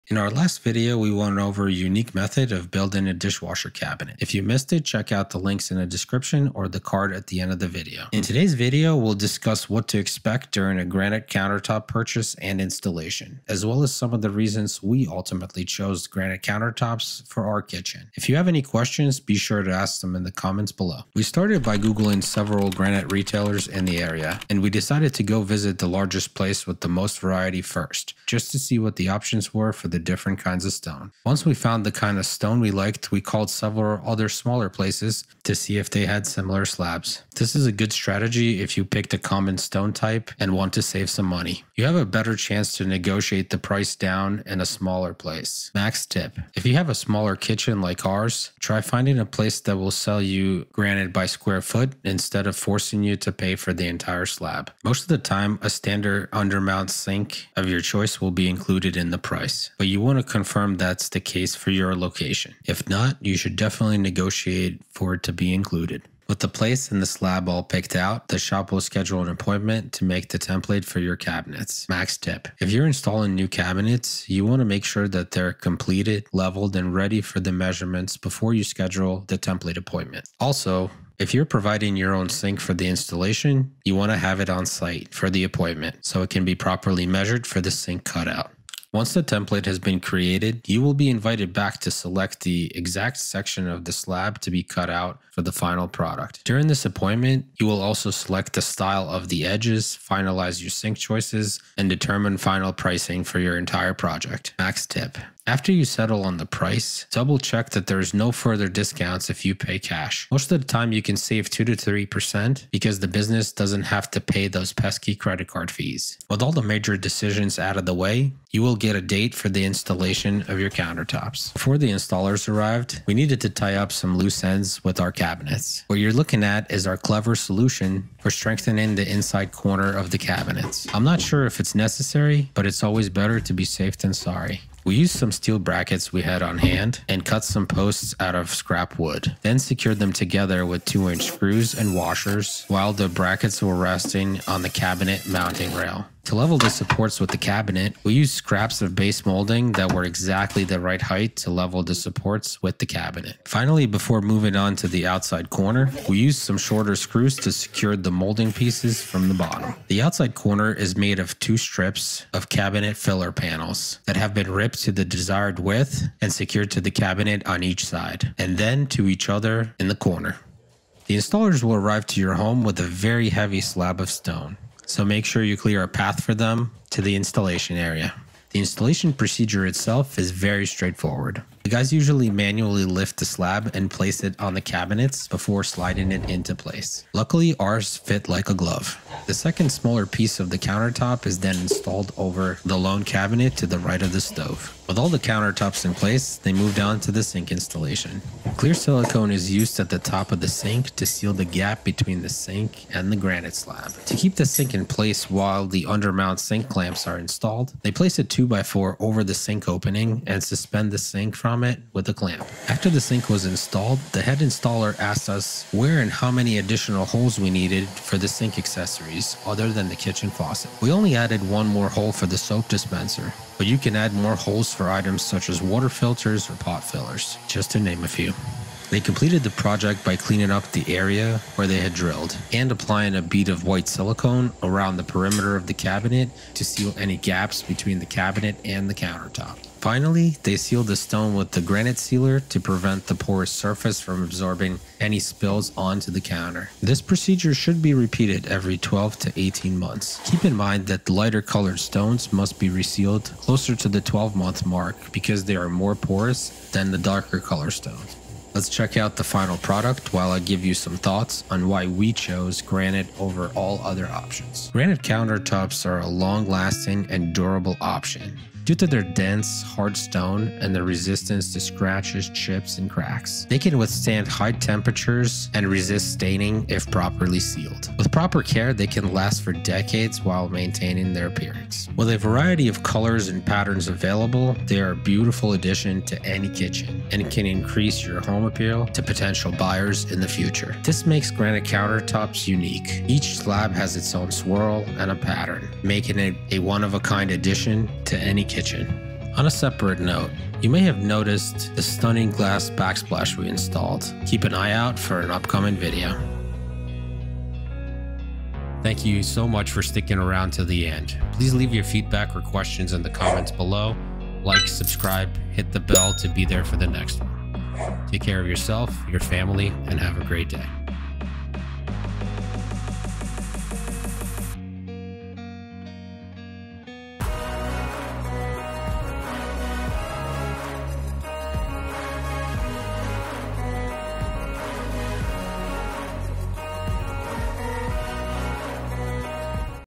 Granite Countertop Installation: What to Expect!
If you're planning to install granite countertops in your home, this video is perfect for you. In this video, we'll show you what to expect during the granite countertop installation process. Our step-by-step guide will walk you through the countertop purchase to maintenance process. We'll also share essential tips and tricks for a successful installation. Additionally, we'll provide maintenance tips to help you keep your natural stone countertop looking great for years to come. So, whether you're planning a kitchen remodel or a home renovation project, this video will provide you with all the information you need to complete your project successfully.

Corner Strengthening:
🛠️ One-Handed Bar Clamps
🛠️ 20V Impact Driver
🛠️ 24-Inch Level

00:00 - Granite Countertop Purchase Process
01:22 - Template Appointment and Selection
02:16 - Cabinet Preparation for Installation
03:15 - Granite Countertop Installation
04:15 - Finishing Touches